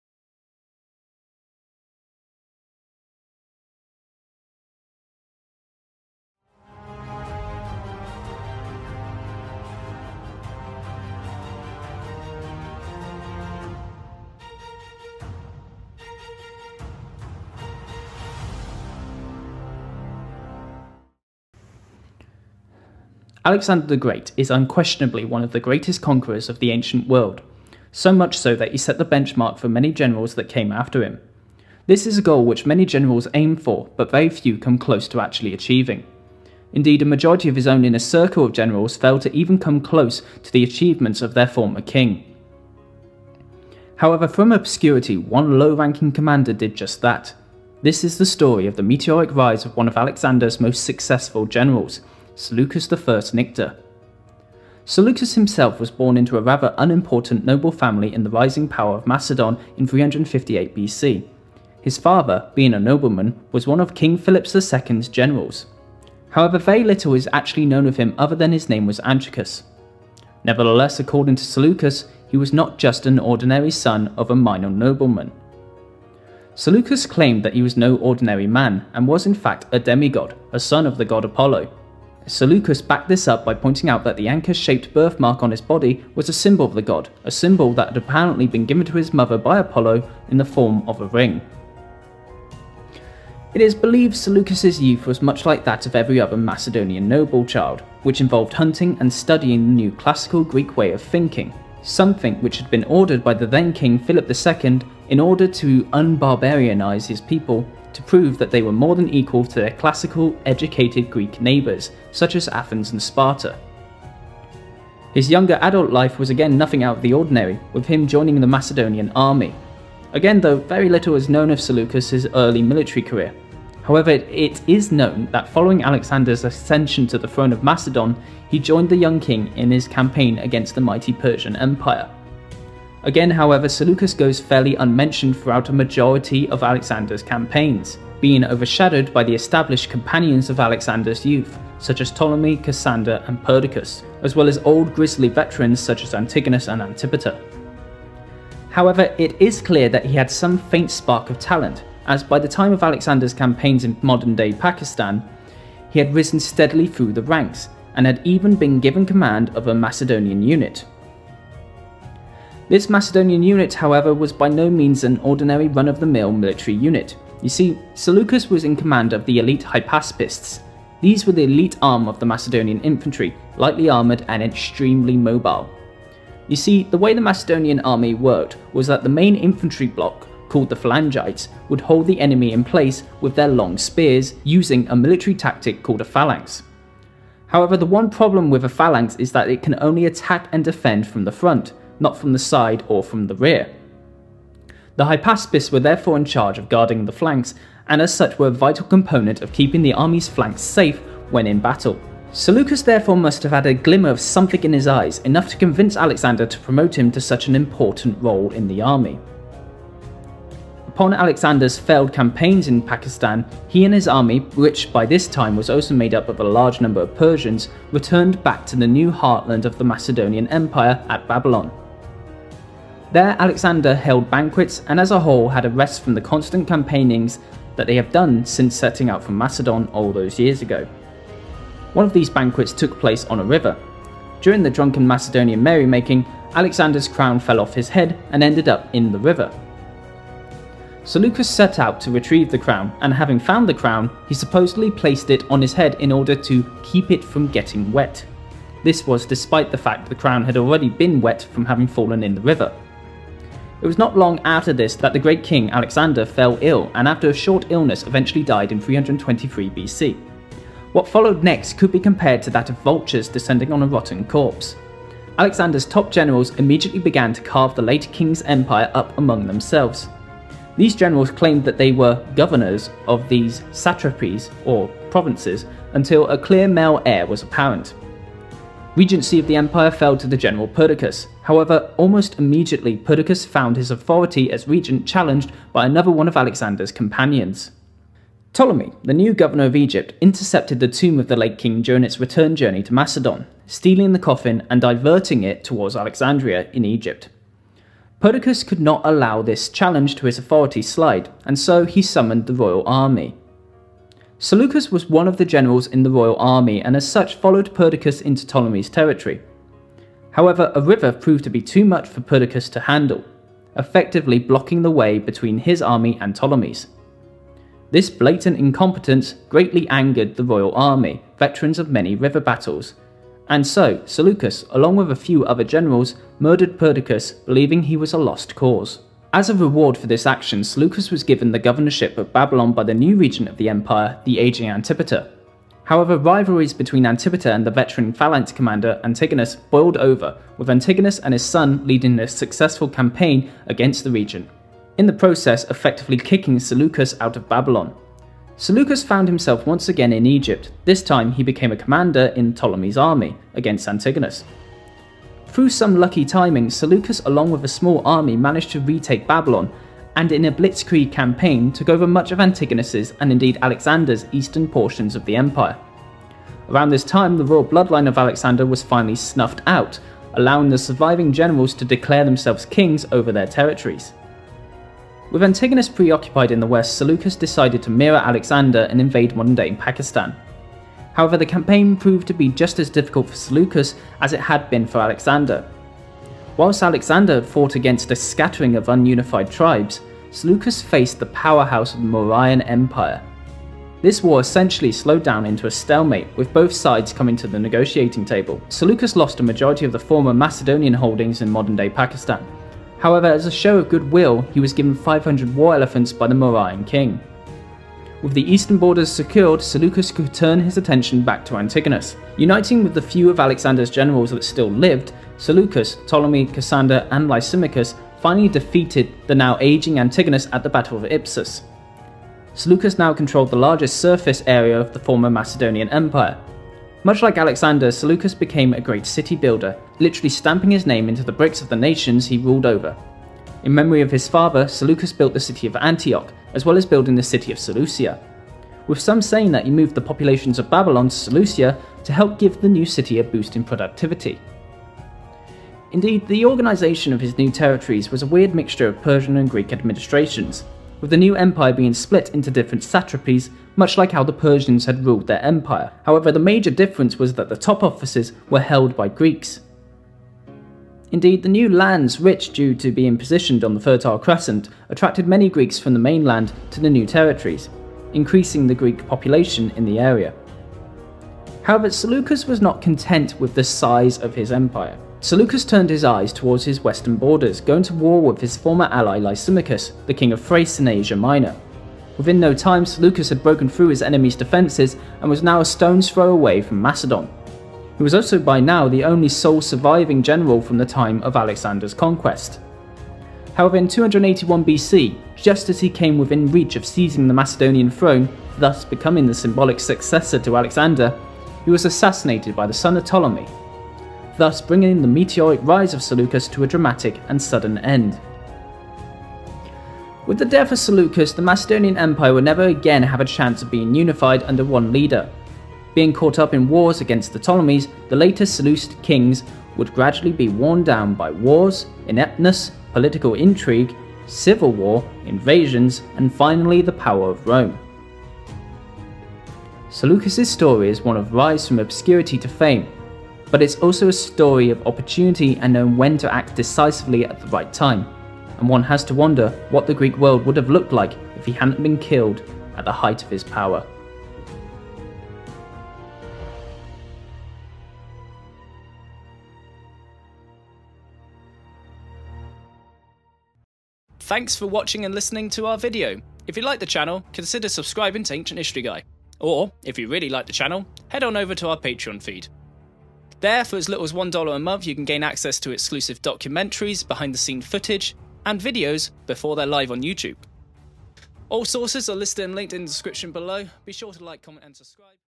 The Last Man Standing: Life of Seleucus I Nicator
How did one low ranking general in Alexander the Great's army, found one of the largest Empires in history? FIND OUT NOW!